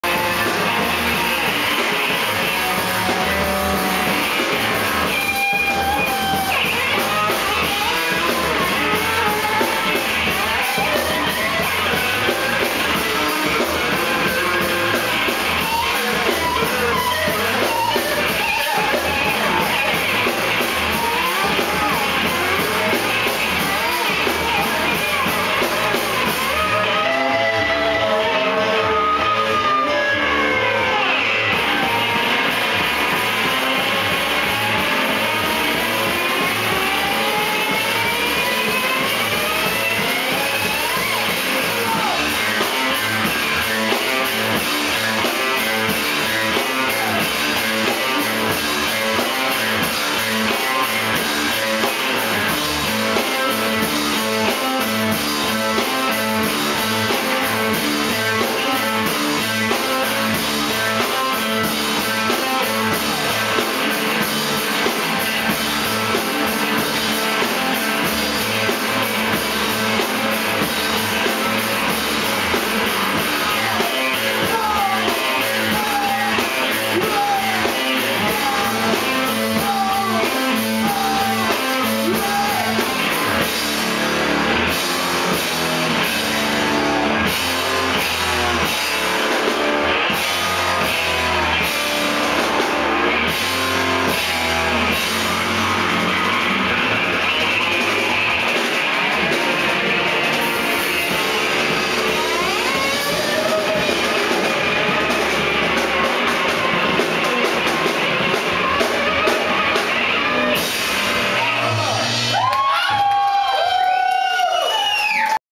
agar agar @ eagle tavern anna conda victory party
Thank you Anna Conda for introducing me to this kick ass two bass trio.  Great, great lead bass, very inspirational!  I have a few more video clips from this show if anyone is interested.